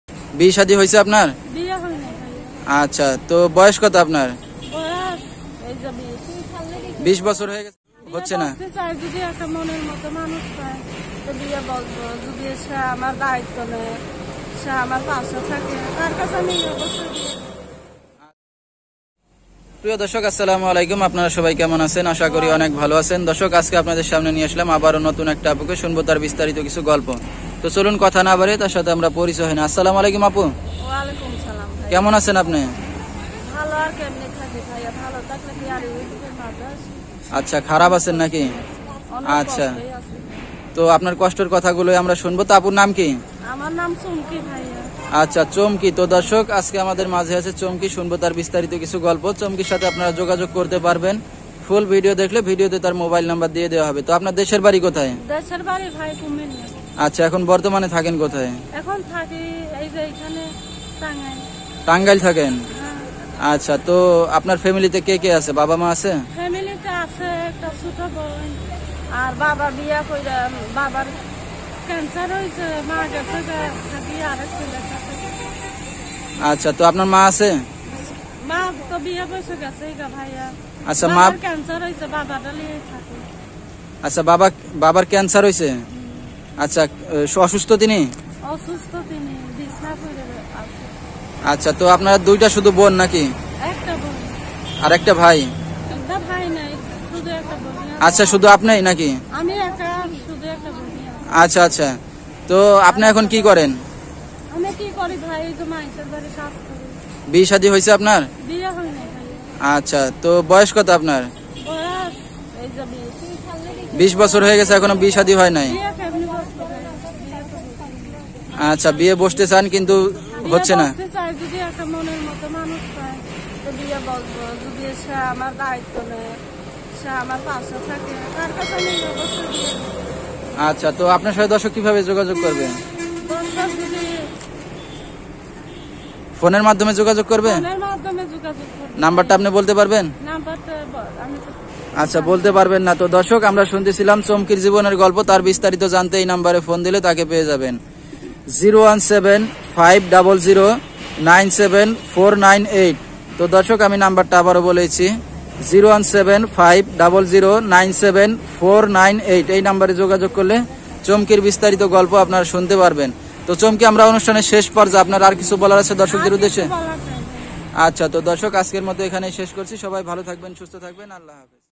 সুন্দরী চুমকি !! আমার মনের মতো যে হবে তাকে আমি সবকিছু উজাড় করে দেবো
প্রিয় দর্শক আসসালামু আলাইকুম আপনারা সবাই কেমন আছেন আশা করি অনেক ভাল আছেন দর্শক আজকের প্রতিবেদন আমরা তুলে ধরেছি চুমকির জীবনের জানা-অজানা কিছু গল্প তার বিস্তারিত জানতে ভিডিওটি দেখুন